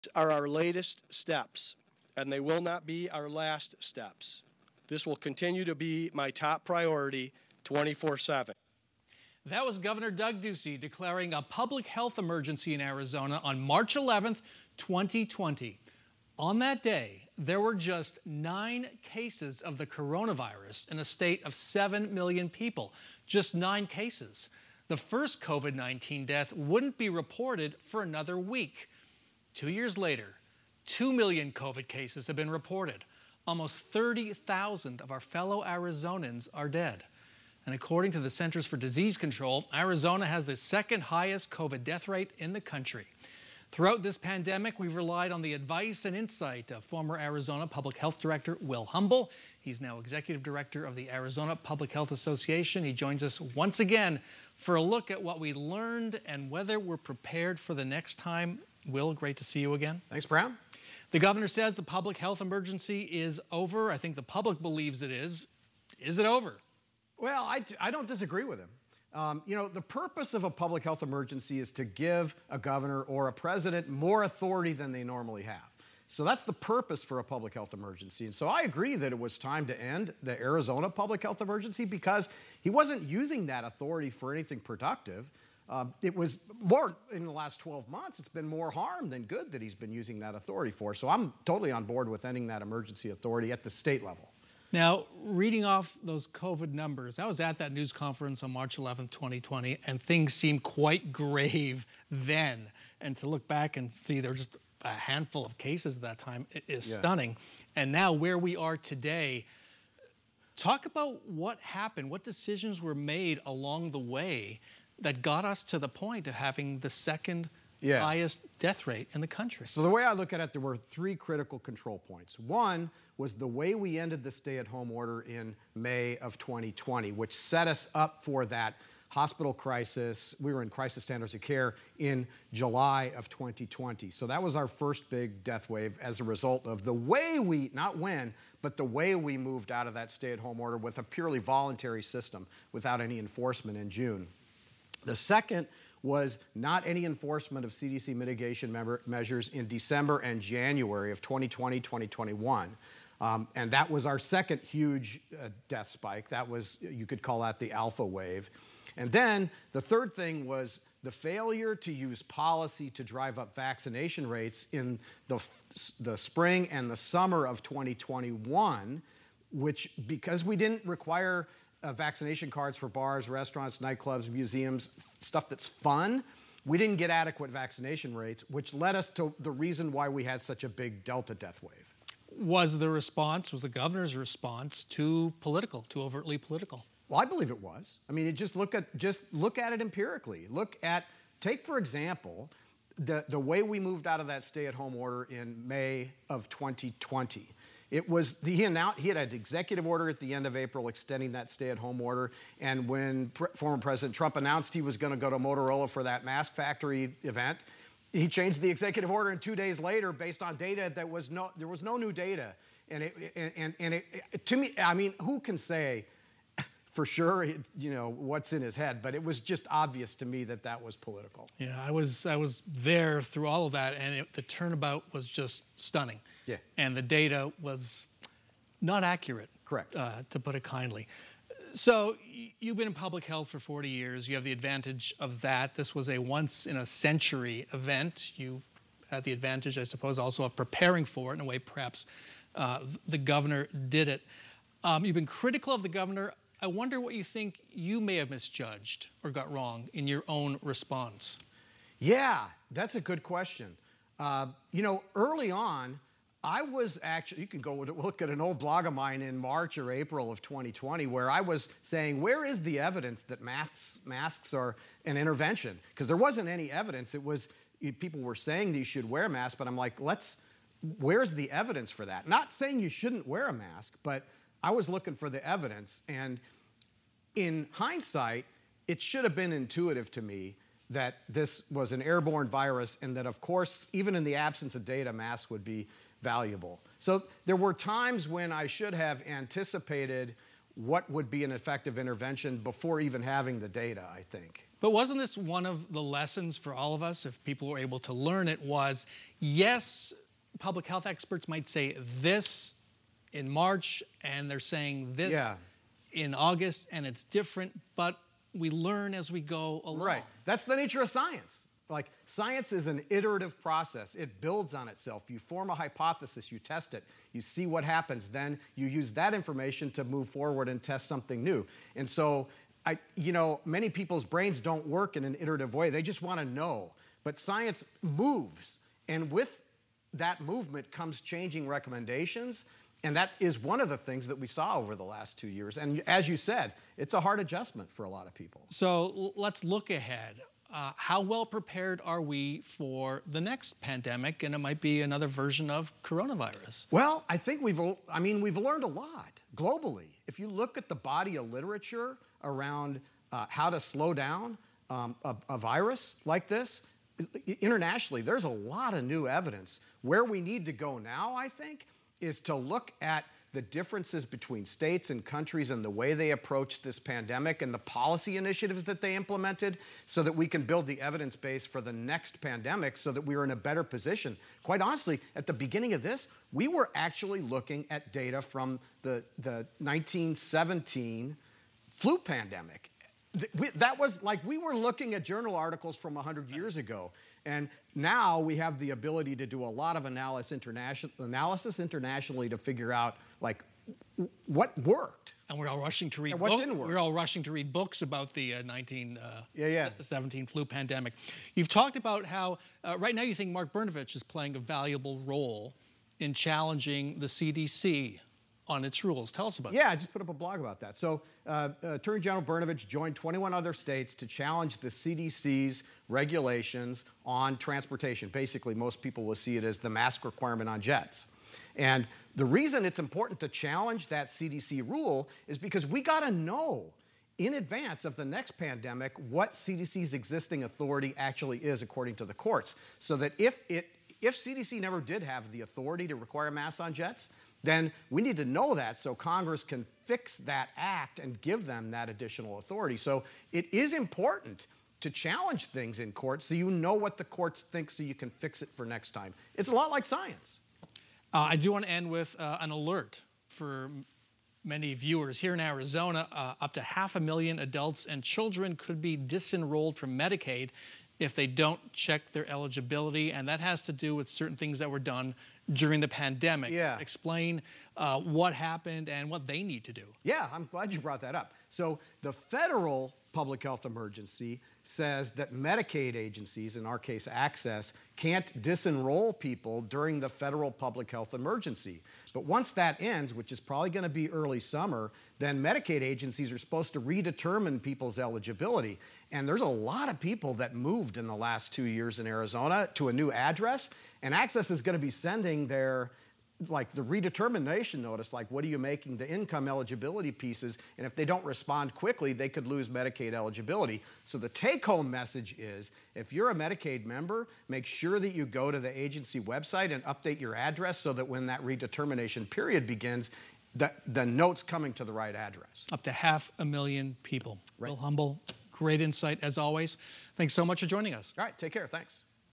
Arizona governor lifts COVID emergency. Is it over?
Former Arizona public health director Will Humble reviews Gov. Doug Ducey's handling of the pandemic over the last two years.

He also looks ahead to what needs to happen now that the public health emergency has been lifted.